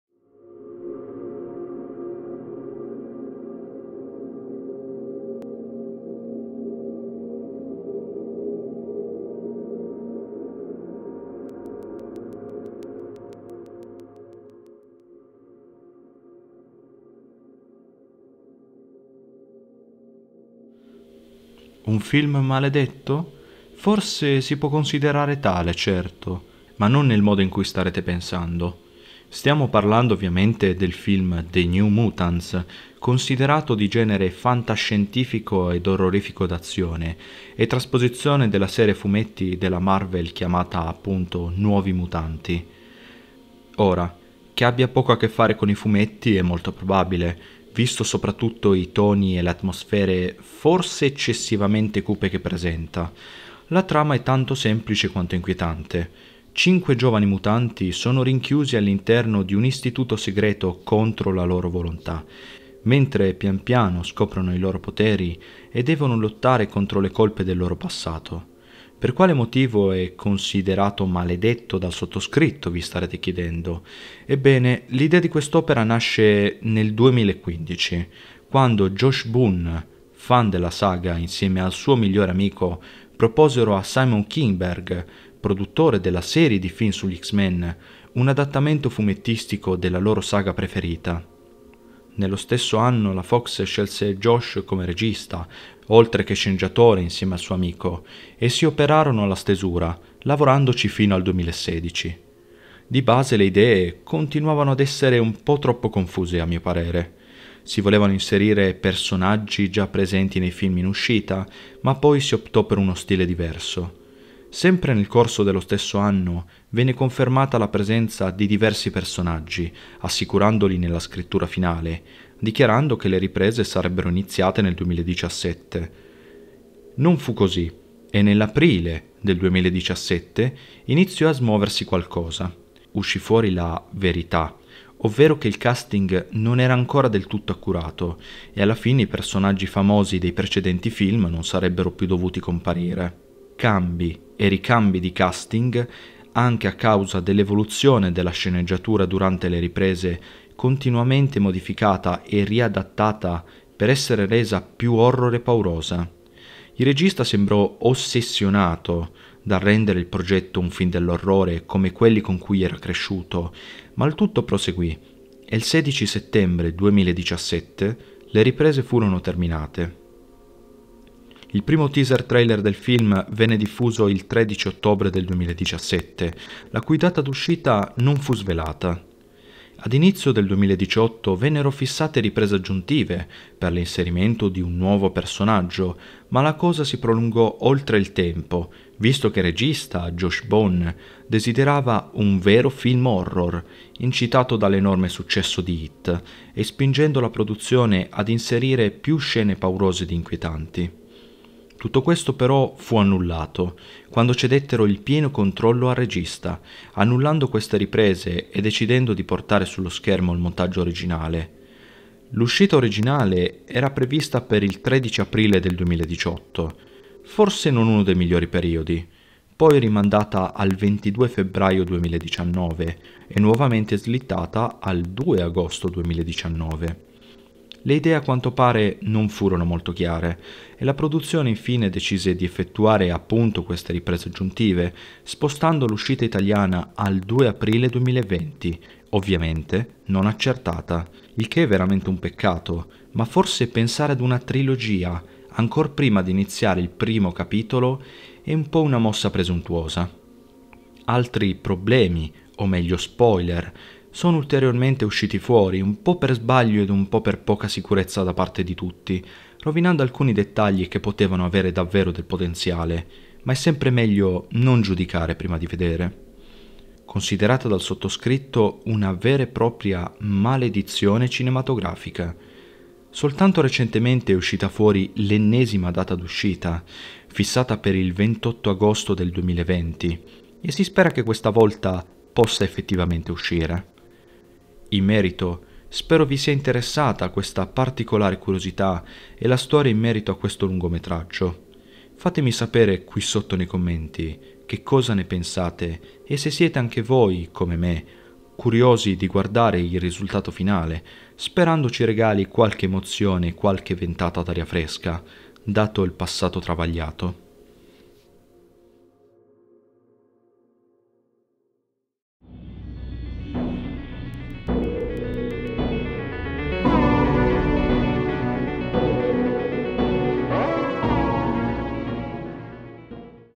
The New Mutants: Un film maledetto?
Un vero e proprio film "maledetto", ma per quale motivo?
Guardate il video per scoprirlo!